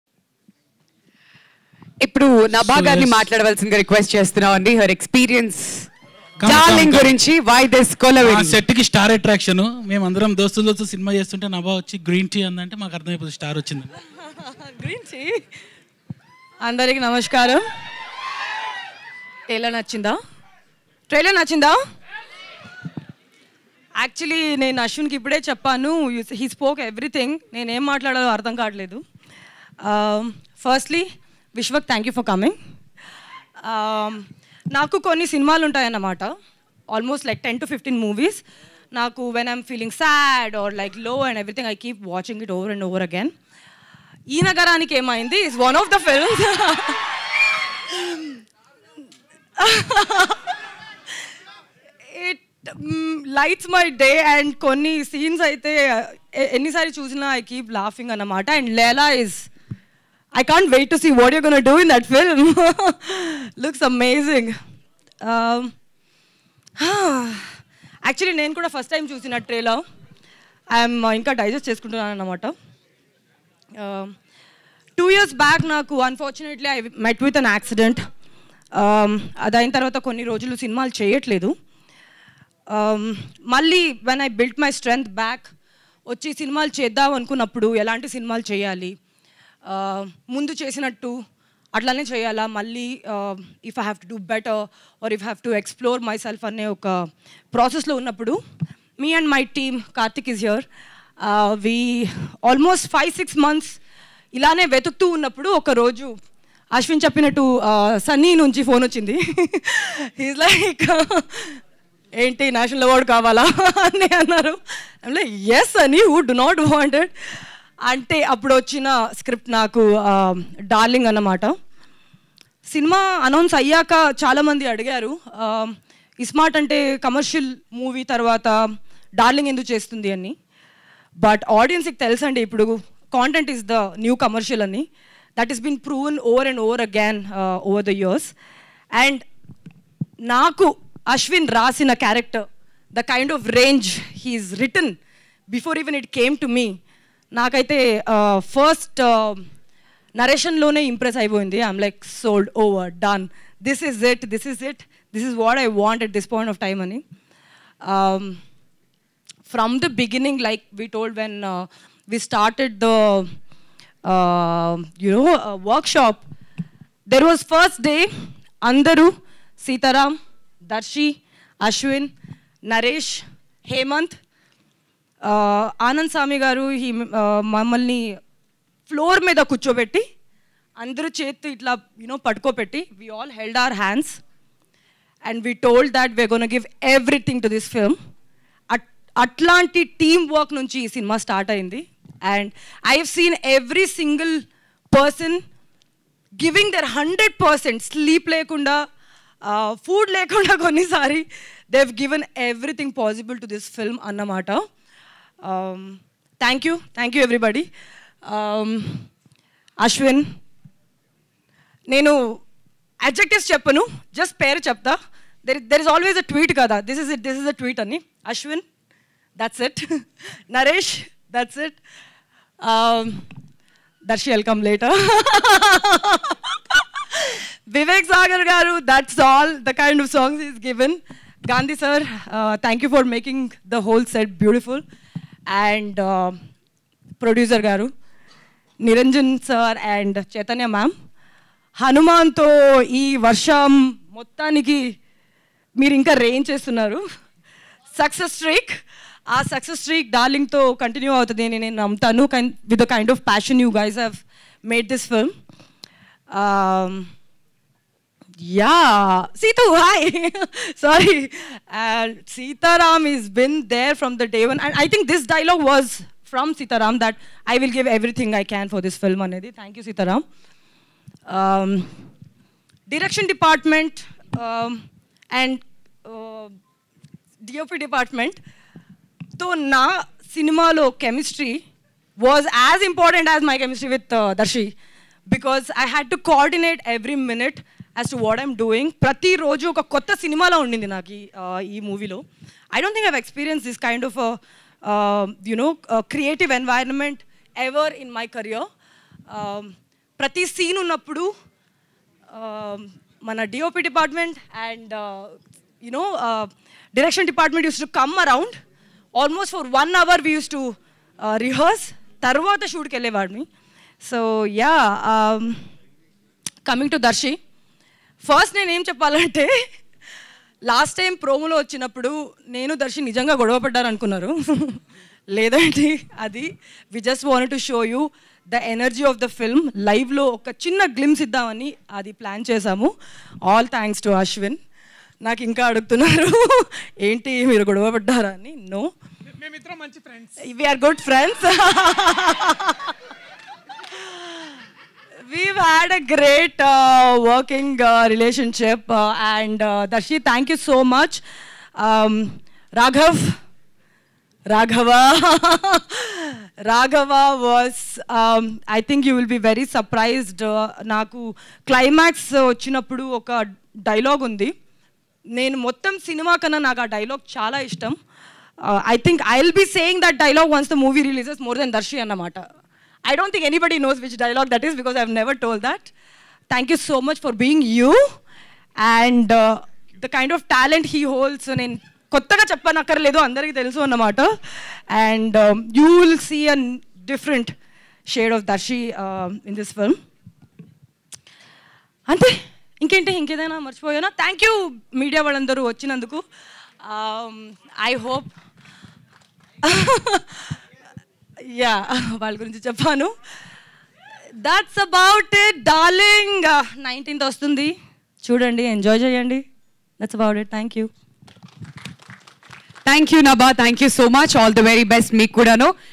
Nabha Natesh Speech at DARLING Movie Trailer Launch | Priyadarshi | Vishwak Sen | Tupaki
WATCH HERE: Nabha Natesh Speech at DARLING Movie Trailer Launch | Priyadarshi | Vishwak Sen | Tupaki